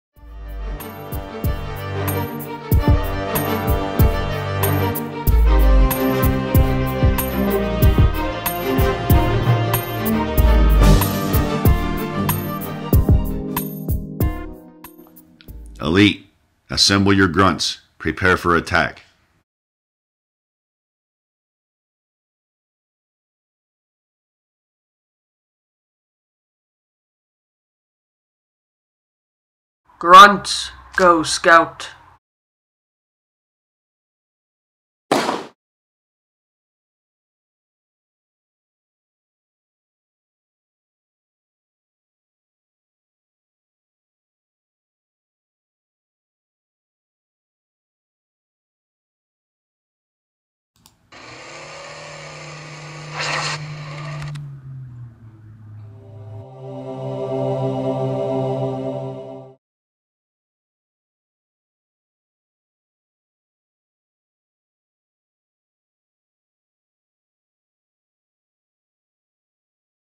Scouting- megabloks halo stop-motion
A short I made with a lot of help from a friend. I used my new camera, which has a much better quality than my old one. From now on I will use my new camera. (The brute chieftain 2 and 3 were filmed with my old camera, so when they are released the quality will not be as good.)